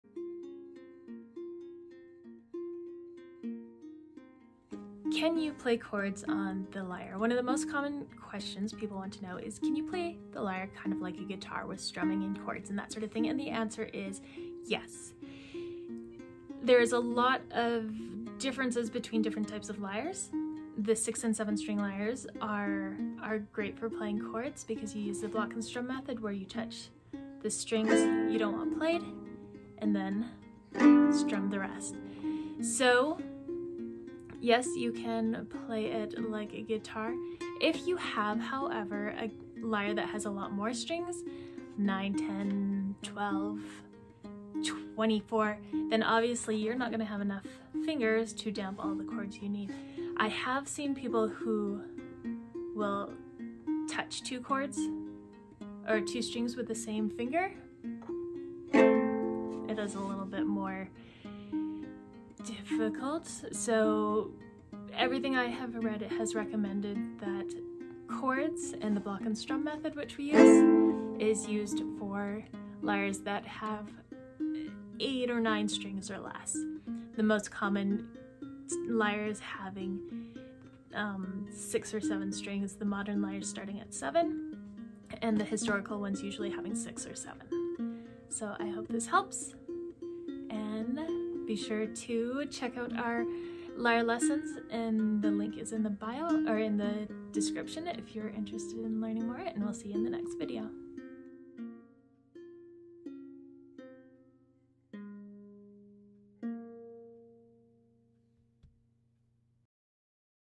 Can You Play Chords on the Lyre?
Ever wondered about playing the lyre like a guitar? You're not alone! Join us to learn about playing chords on the lyre.